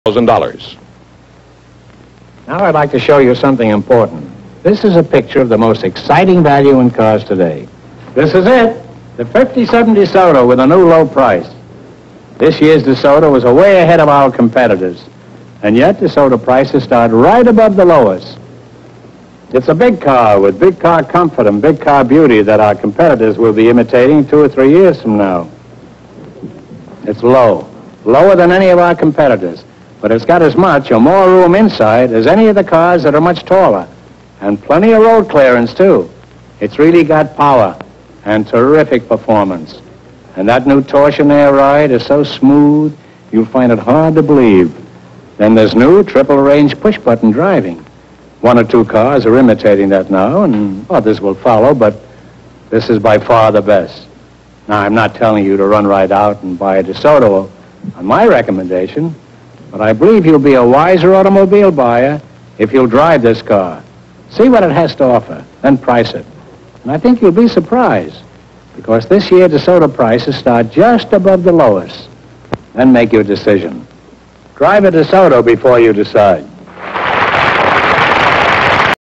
Vintage commercials - DeSoto automobile 1950's
DeSoto automobile commercial, featuring Groucho Marx.